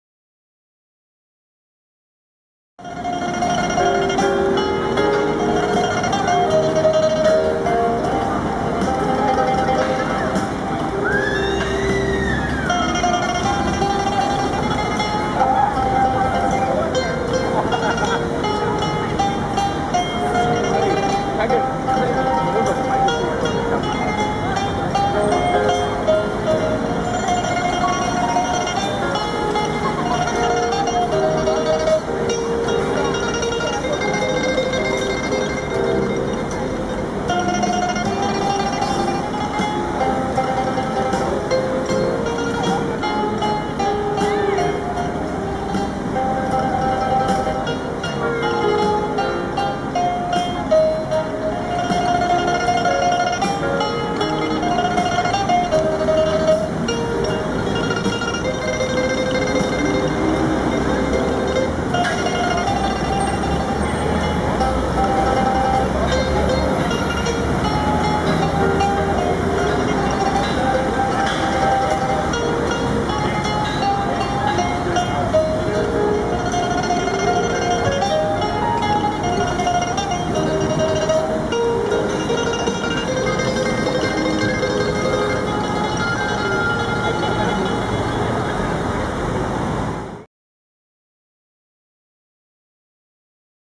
Singapore Street Music
Music at the Singapore national holiday.